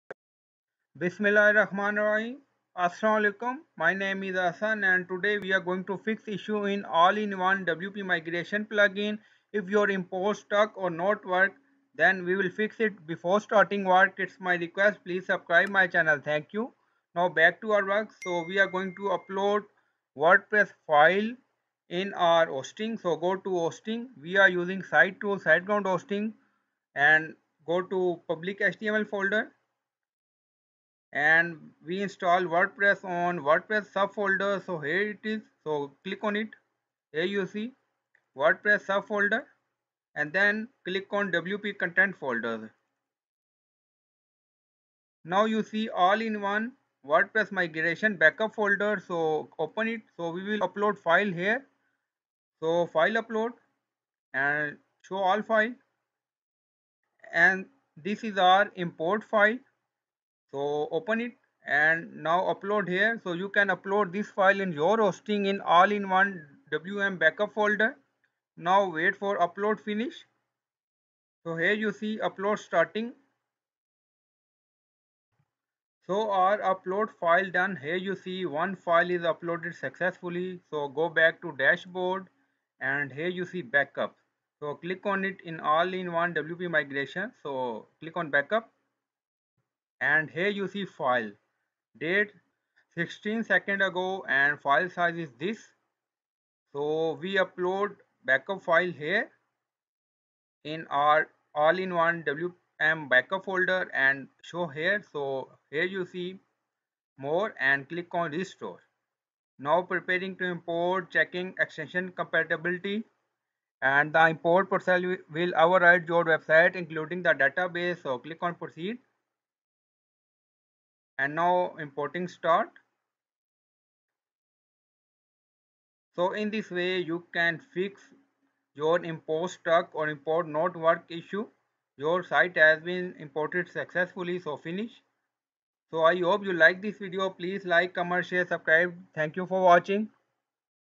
How to Fix WordPress Migration Import Stuck and Not working in All In One WP Migration Pro - Solved
In this wordpress tutorial you will learn how to fix issue while importing wordpress file in all in one wp migration plugin. We are using all in one wp migration pro unlimited extension plugin. While Wordpress File Migrate in new domain then Import stuck and not work problem occur then you will upload file in hosting in plugin folder to fixed this.